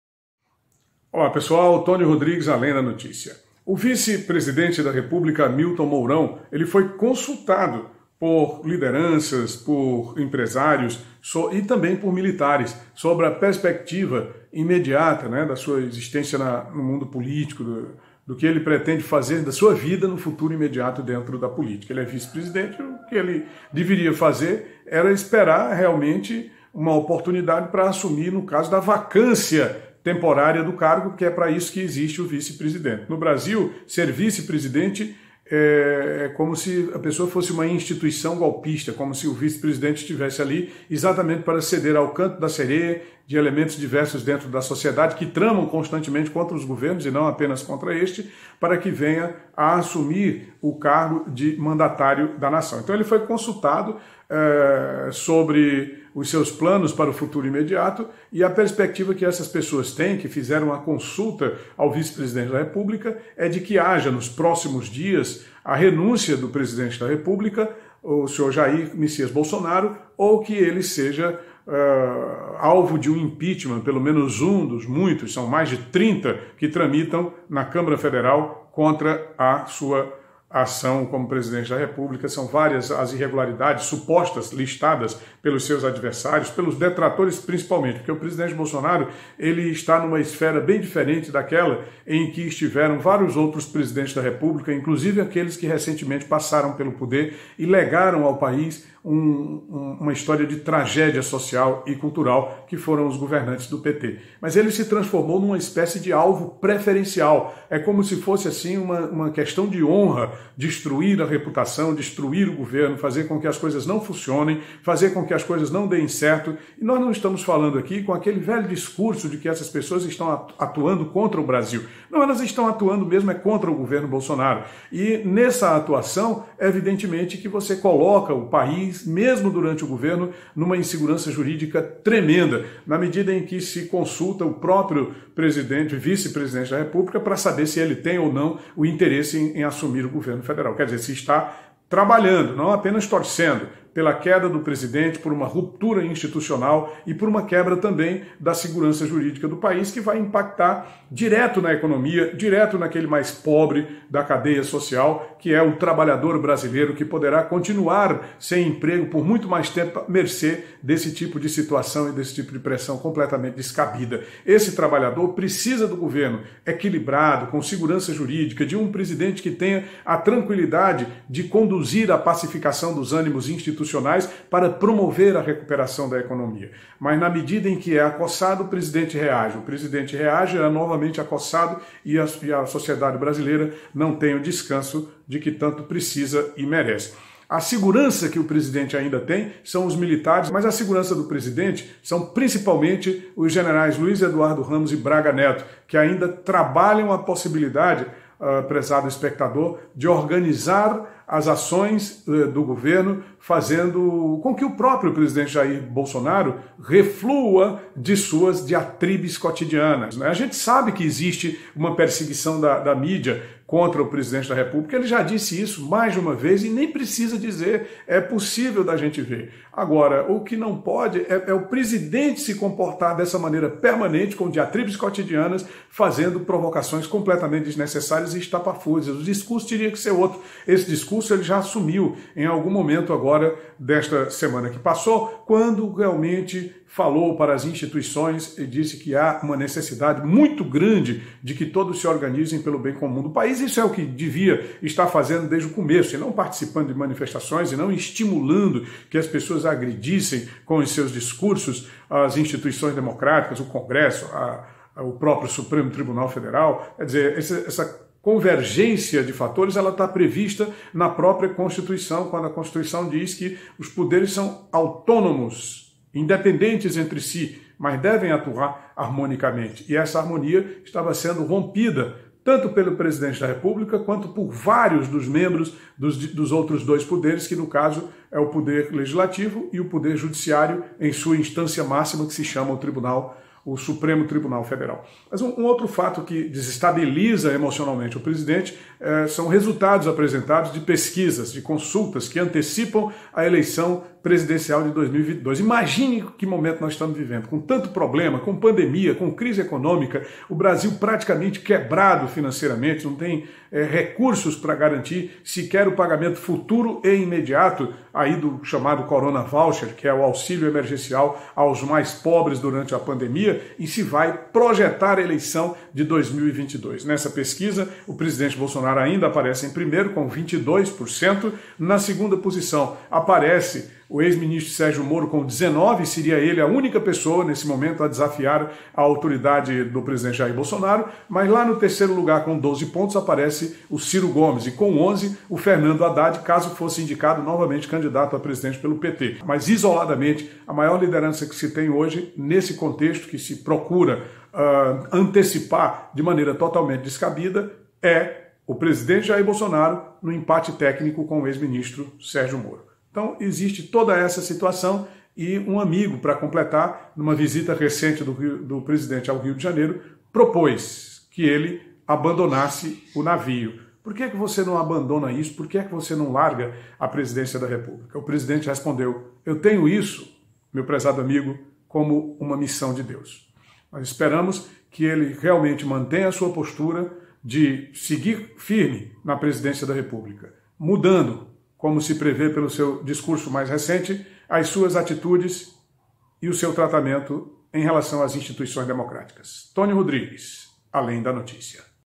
Mourão presidente
✔ Um amigo do presidente Bolsonaro perguntou a ele: por que não renuncia? Ele respondeu: porque tenho uma missão. O vice Mourão foi consultado: aceitaria substituir Bolsonaro? Ao Mourão foram postas duas alternativas: presidente pode renunciar. Ou então pode sofrer impeachment. Existem na Câmara mais de 30 pedidos de impeachment contra Bolsonaro. Mourão ficou em silêncio. Pelo menos até agora. De outro lado, já se divulga pesquisas sobre a eleição de 2022. Antecipar a eleição não é bom para o governo e muito menos para o país. O governo precisa de segurança. O país precisa do governo. O povo carece de todos.